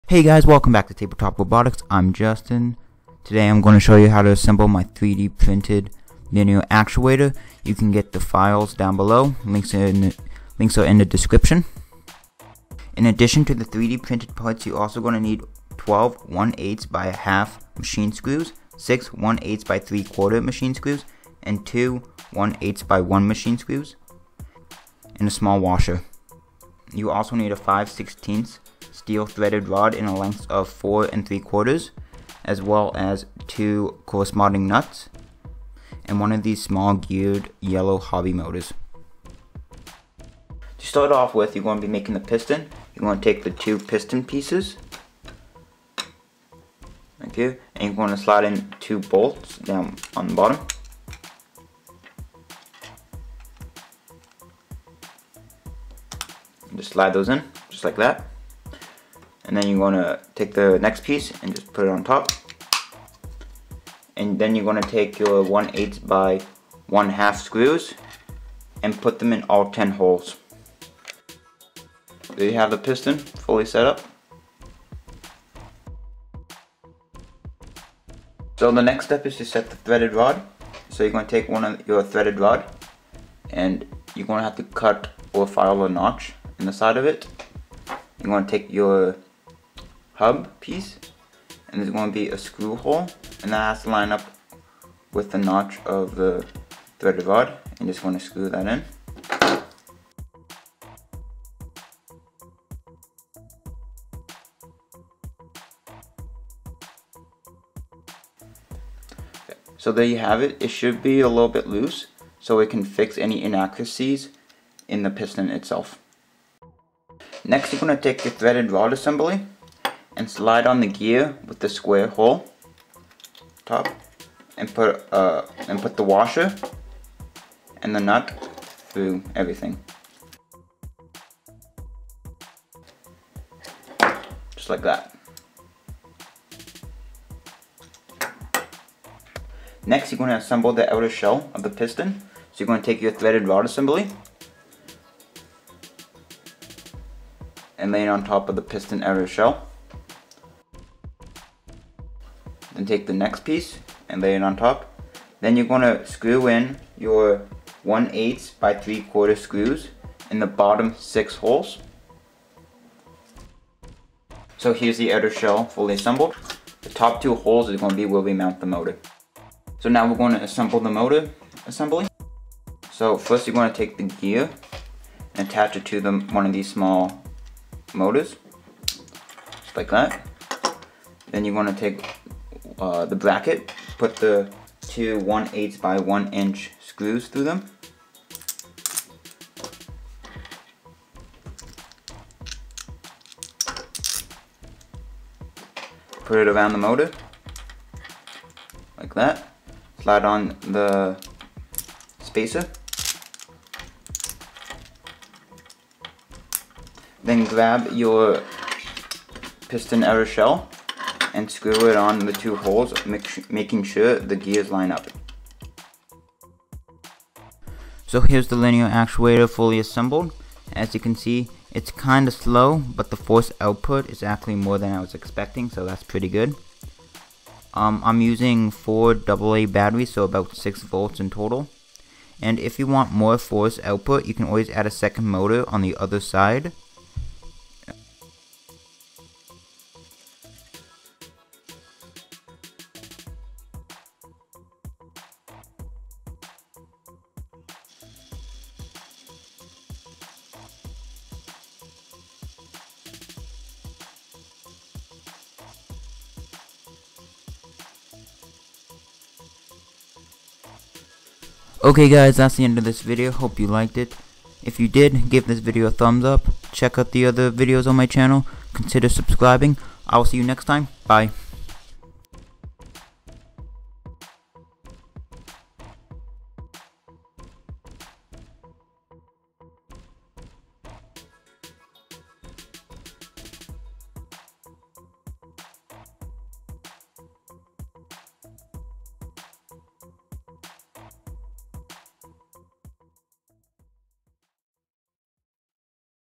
3D printed linear actuator build
This is a simple, easy to build, linear actuator. I designed the actuator so that supports are not needed,

other things you need:
-12(1/8x1/2 machine screws)
-6(1/8x3/4 machine screws)
-2(1/8x1 machine screws)
-1 washer that fits on the screws
-1 (5/16 steel threaded rod length of 4 3/4)
-2 nuts that fits on the threaded rod
-yellow geared hobby motors